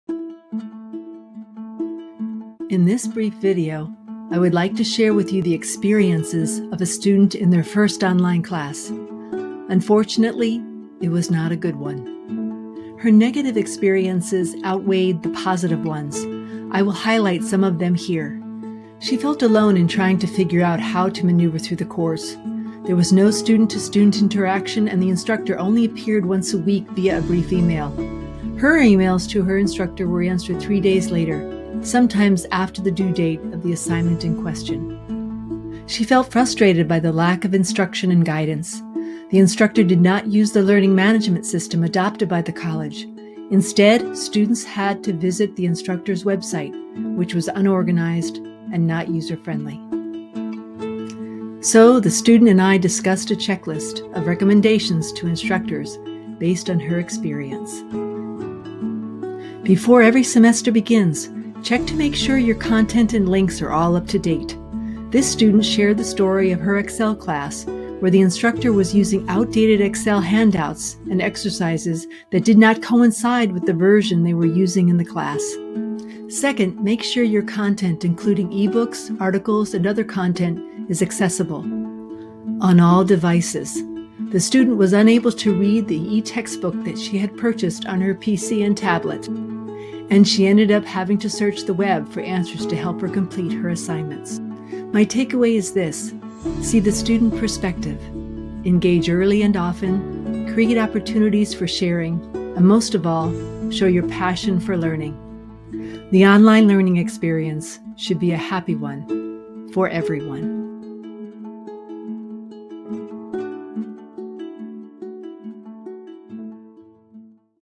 Online Student Voices
My interview with a student about her online learning experience. We came up with some suggestions that instructors can follow as they prepare for a new semester.